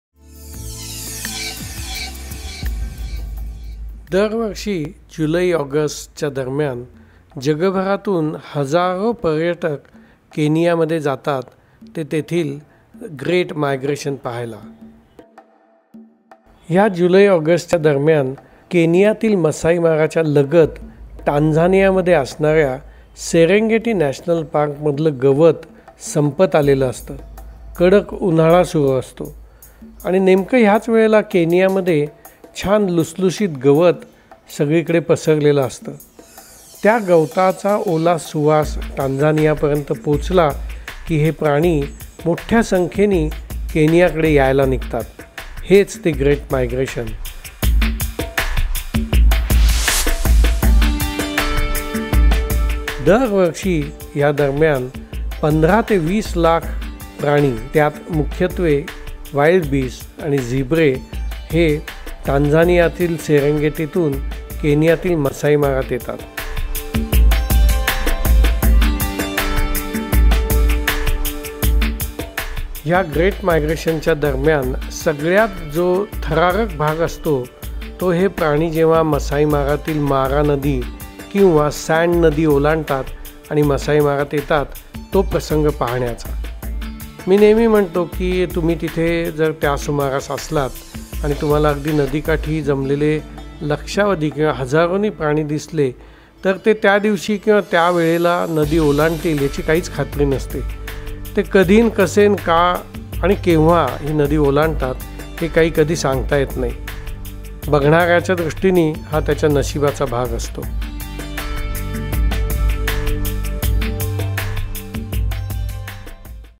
Kenya Migration 2015
Migration of the wildebeest and other animals from Serengeti into Mara and back is a phenomenon worth experiencing. DamleSafaris​ takes enthusiasts every year on this nature's pilgrimage